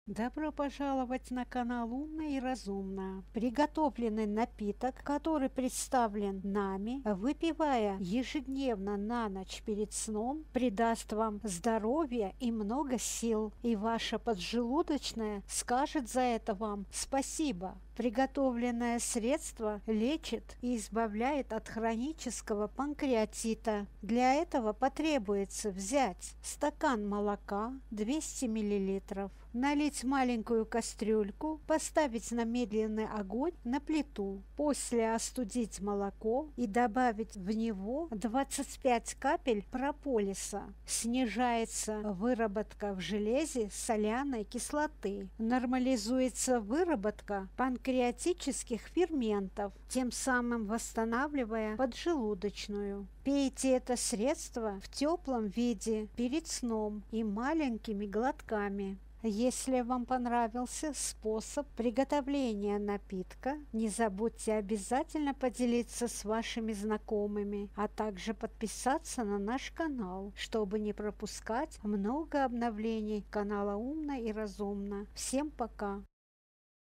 Поджелудочная скажет вам спасибо, выпивая перед сном даст здоровья и силы
Выпивая перед сном на ночь, это средство приготовленное вами, очень простое и быстрое в приготовлении тогда ваша  поджелудочная будет благодарна вам за это, даря здоровье и силу вам...

на трейлере канала  Умно и Разумно в конечных заставках
или